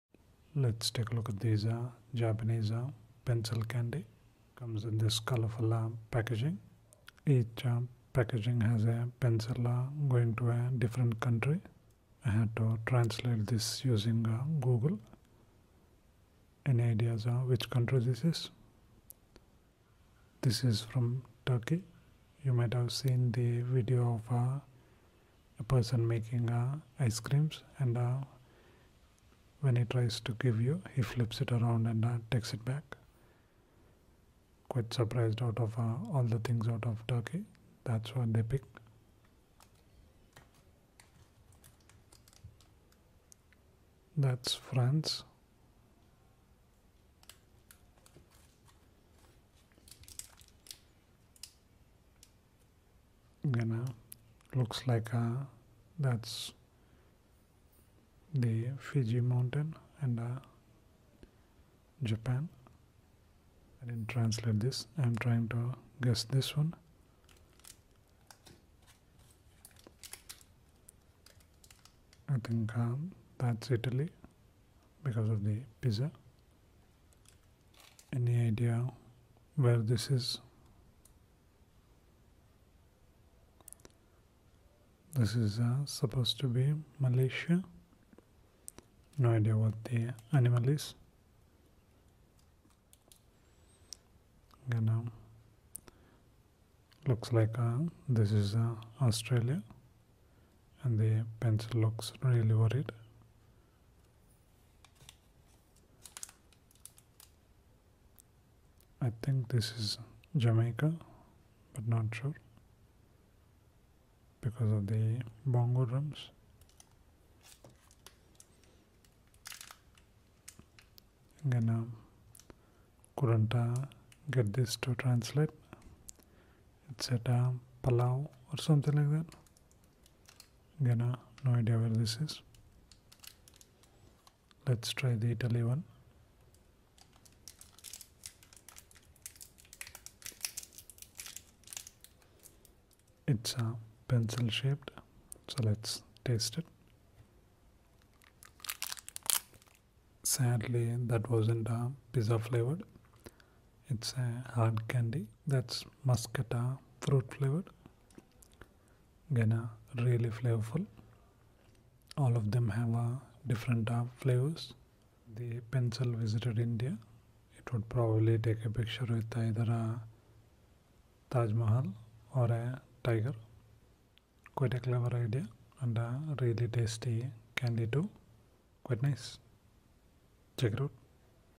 Kanro Pencil Candy
This Kanro Pencil Candy has interesting packaging with the pencil mascot in different countries. Very flavourful candy.

Got Kanro Pencil Candy from Japan